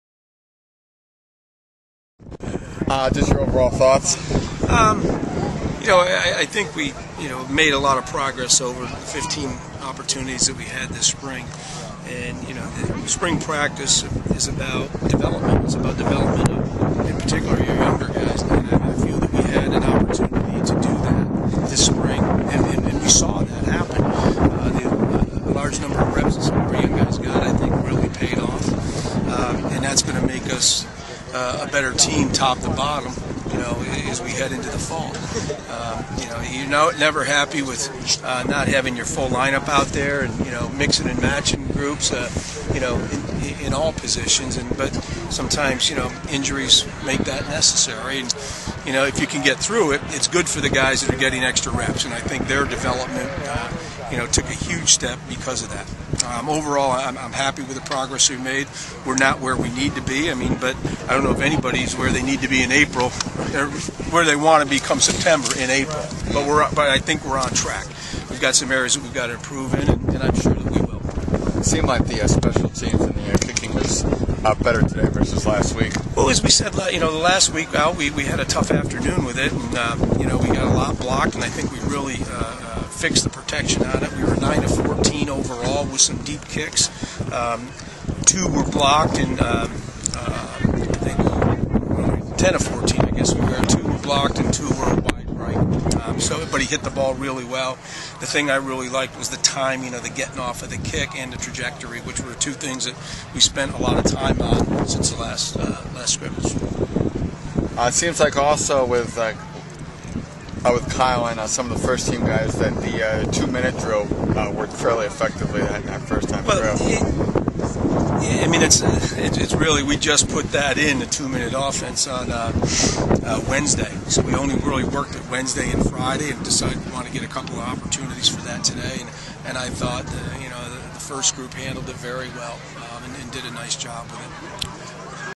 Coach Callahan Comments Post-Spring Game 2011
Monmouth Head Coach Kevin Callahan Comments on the 2011 Spring Game.  Includes overall quotes plus kicker Eric Spillane and the two-minute offense